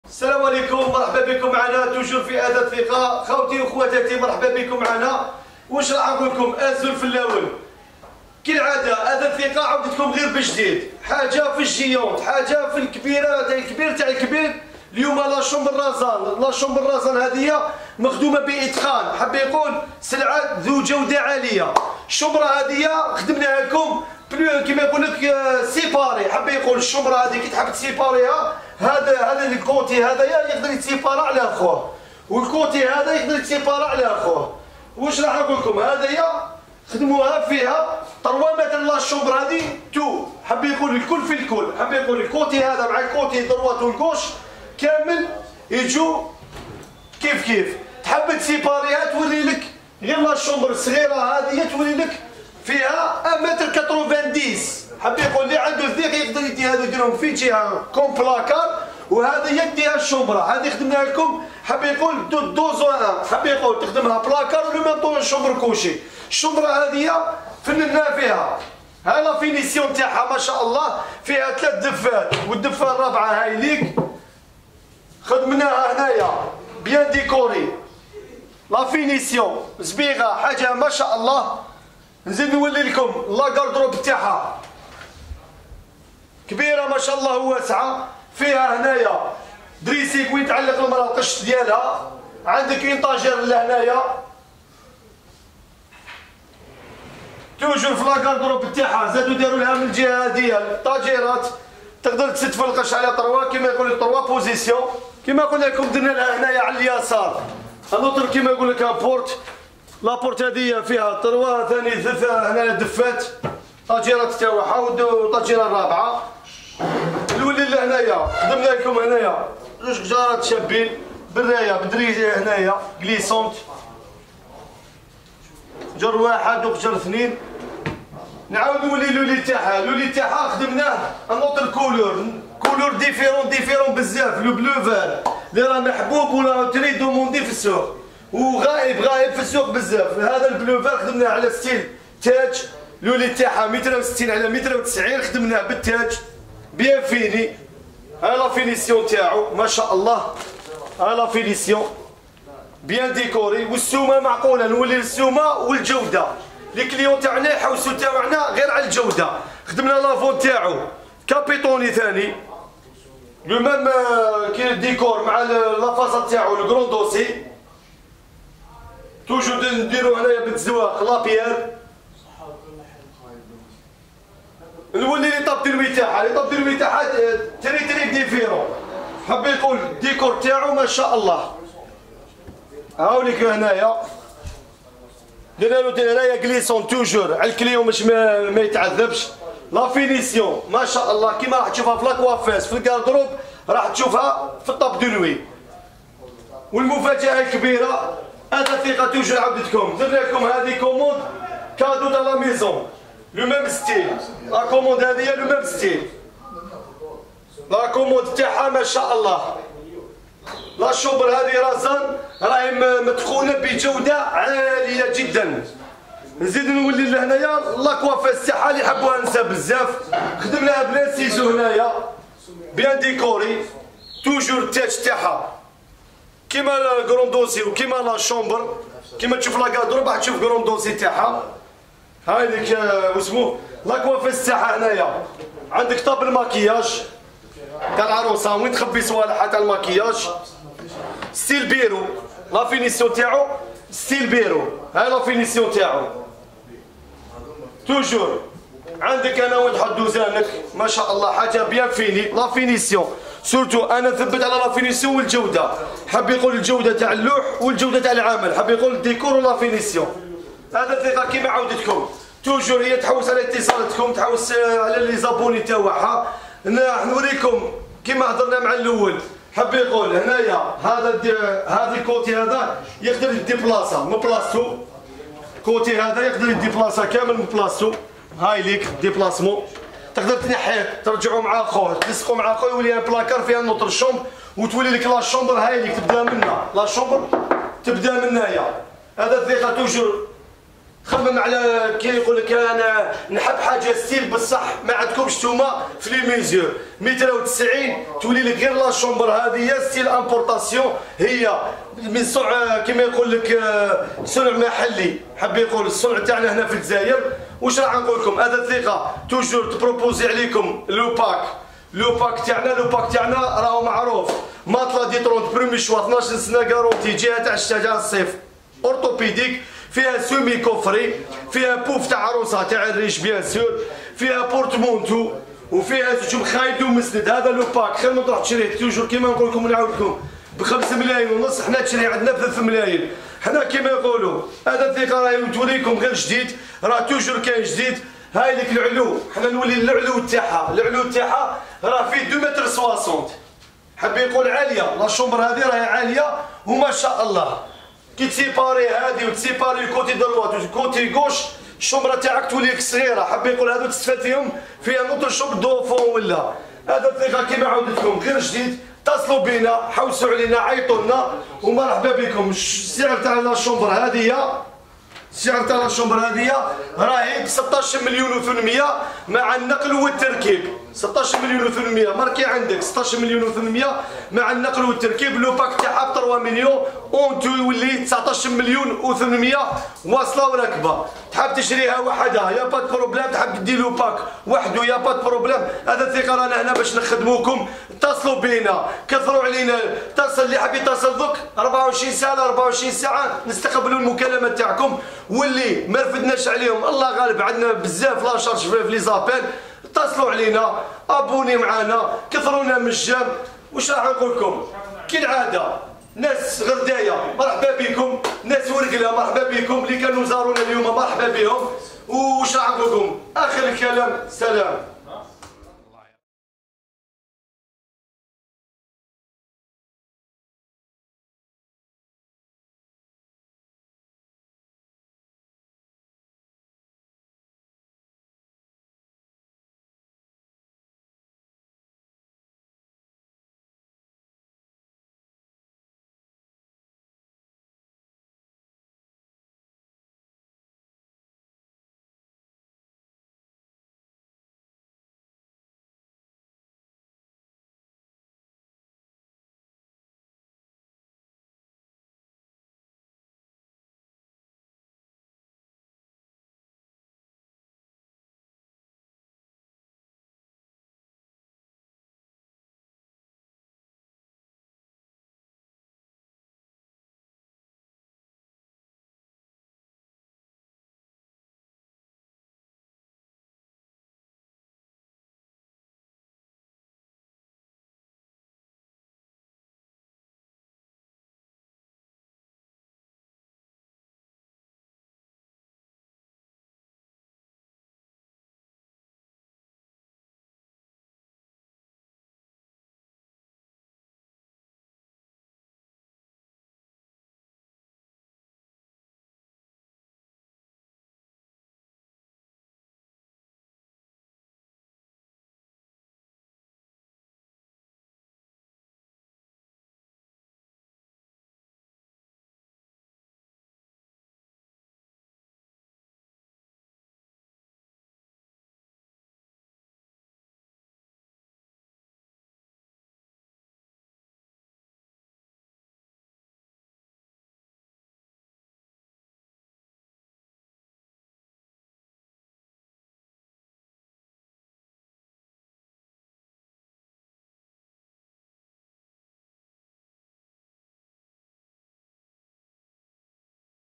أثاث القليعة-غرفة نوم بيضاء من أفخم الأثاث خشب أحمر من صنع جزائري موديل رزان عند أثاث الثقة
خدمة النقل و التركيب مجانية  لـ 48 ولاية 😍❤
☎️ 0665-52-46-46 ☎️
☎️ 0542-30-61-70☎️